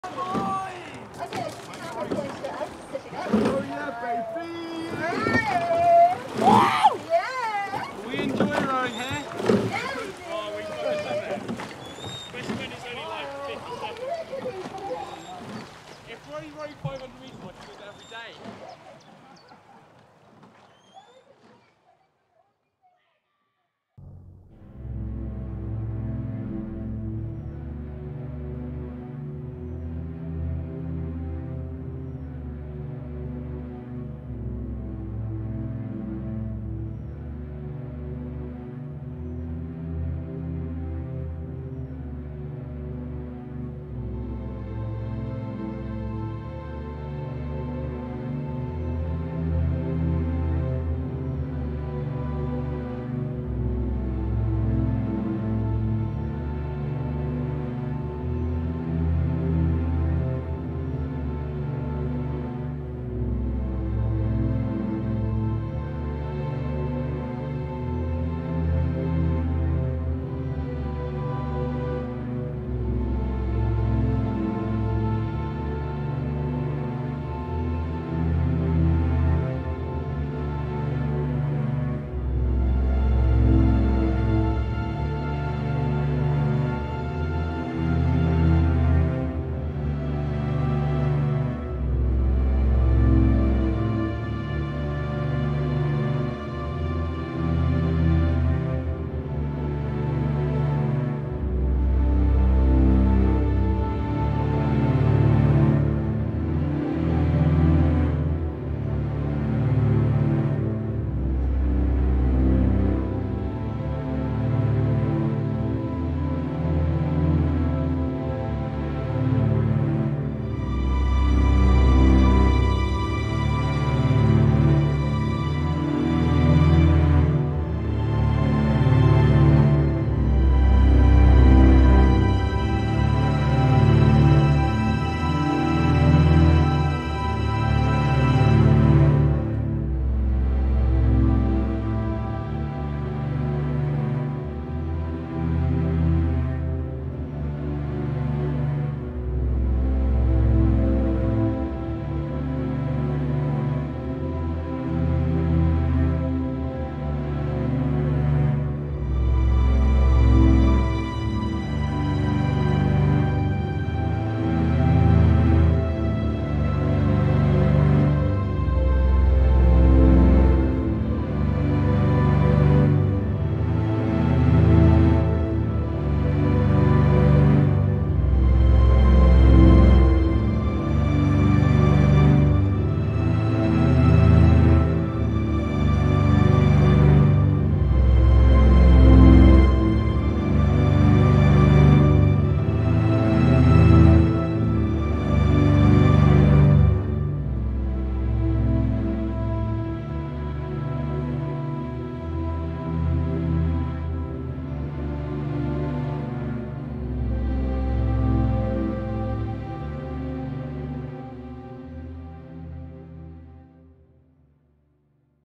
Selwyn M1, Division 1, Thursday, Cambridge May Bumps 2023 slow motion
Footage from the Cambridge May Bumps 2023, a rowing event in Cambridge, UK.